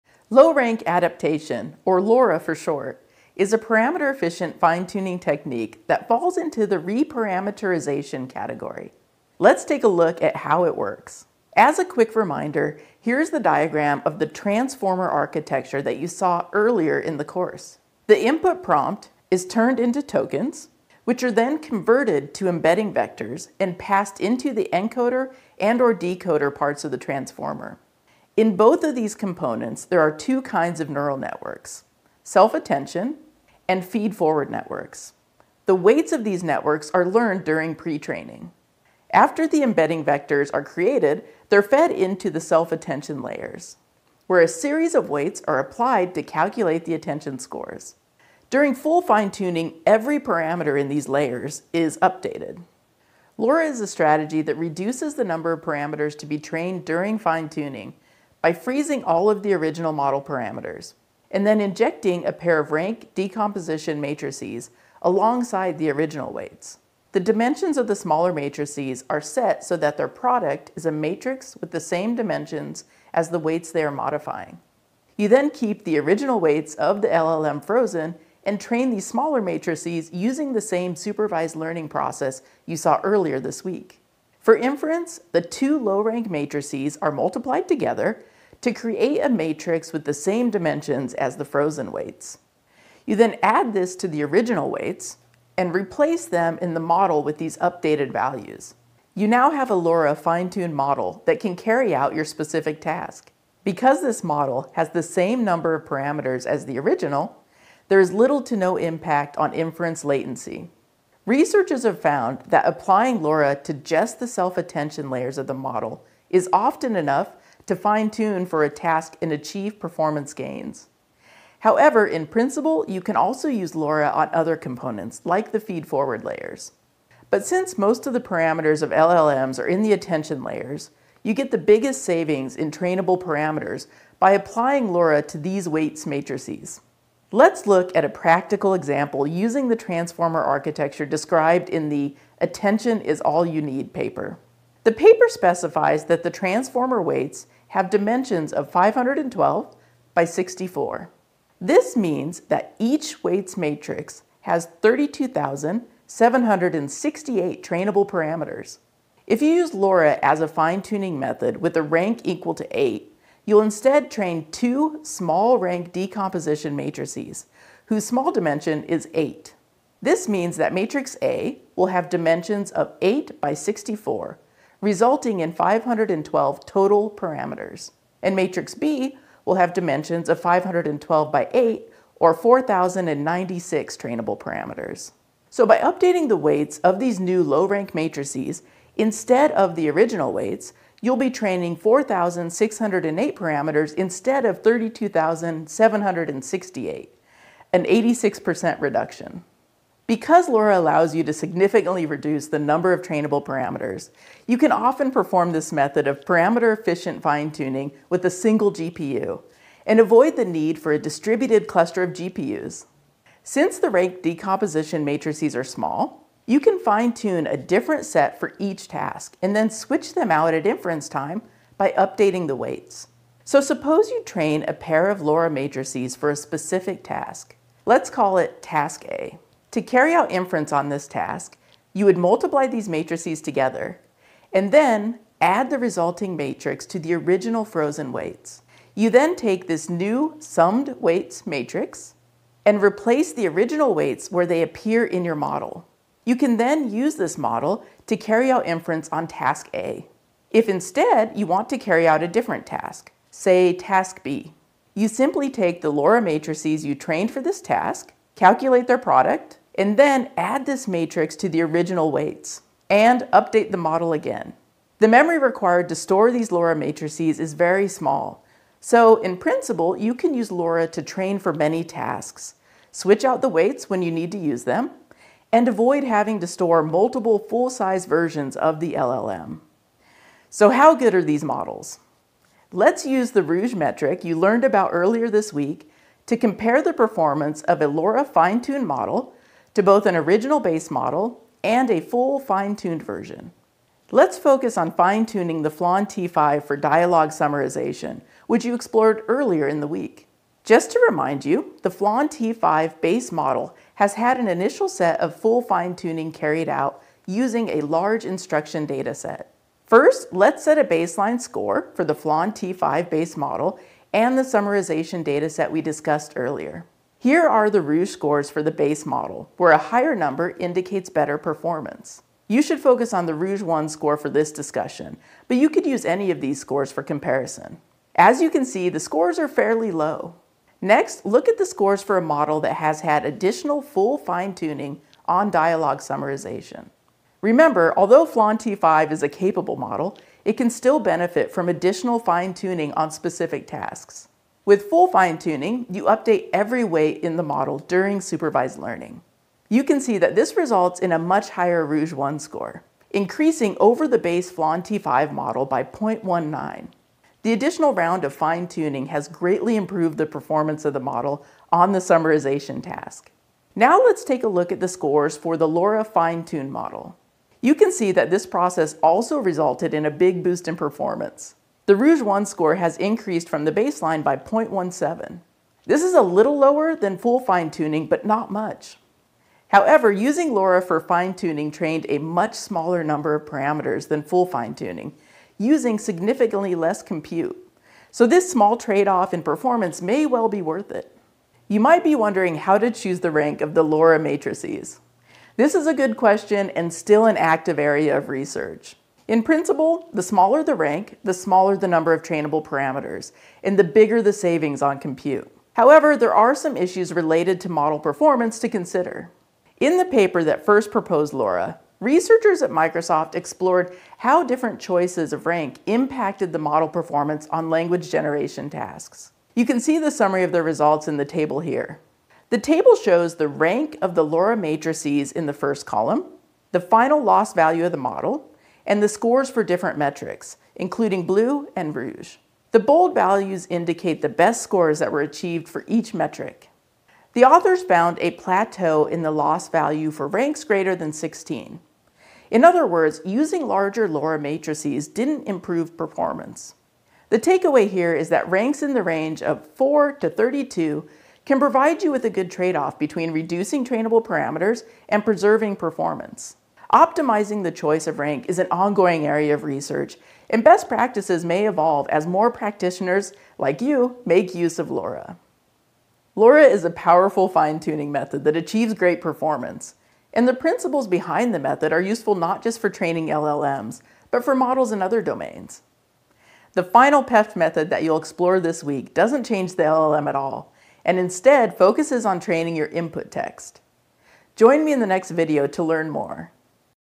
大型语言模型与生成式AI——参数高效微调2——PEFT技术1 - LoRA（低秩适应）
在这个视频中，我们将深入探讨一种名为低秩适应（LoRA）的参数高效微调技术。LoRA通过冻结所有原始模型参数，然后注入一对秩分解矩阵，从而减少在微调期间需要训练的参数数量。

我们将详细解释LoRA如何在变压器架构中工作，包括输入提示的转换、自注意力和前馈网络的角色，以及在完全微调期间如何更新这些层中的每个参数。

此外，我们还将展示如何使用LoRA进行微调，以及这种方法如何减少训练参数的数量，从而使您可以使用单个GPU执行参数高效微调，避免需要分布式GPU集群。

我们还将探讨如何选择LoRA矩阵的秩，这是一个活跃的研究领域。我们将分享一些研究发现，包括秩的选择如何影响模型在语言生成任务上的性能。

最后，我们将使用ROUGE指标，比较LoRA微调模型、原始基础模型和完全微调版本的性能。我们将关注对FLAN-T5进行对话摘要的微调，展示LoRA如何提高模型在摘要任务上的性能。

加入我们，一起探索这种强大的微调方法，了解它如何实现出色的性能，以及它的原理如何对其他领域的模型有用。